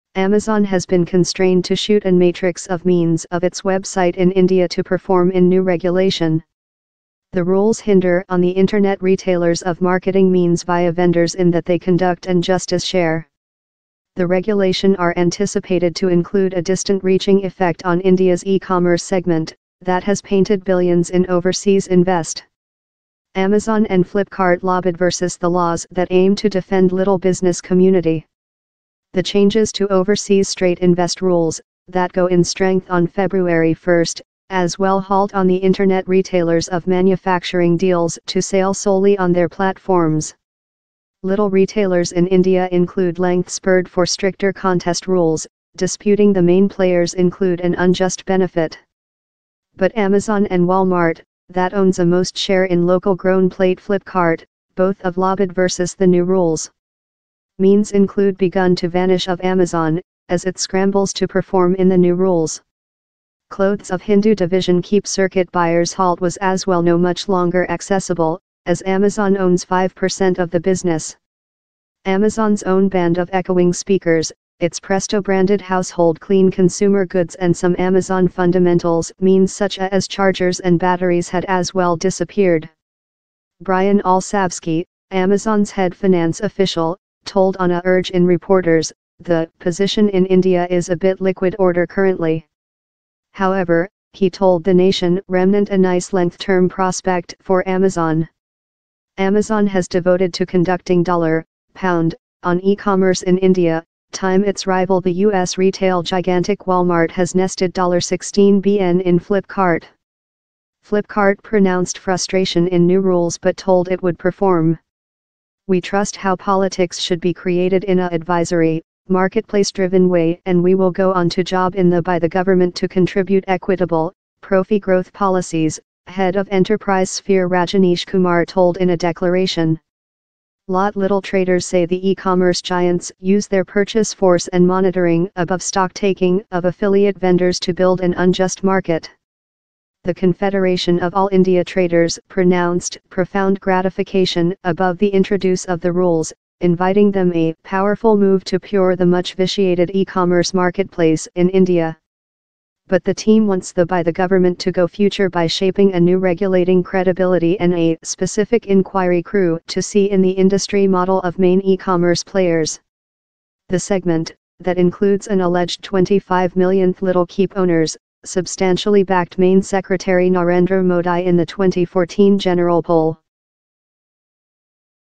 Amazon Constrained To Drag Means In India As New Rules Sting
The e-commerce giant has had to remove products from its India site to comply with new rules.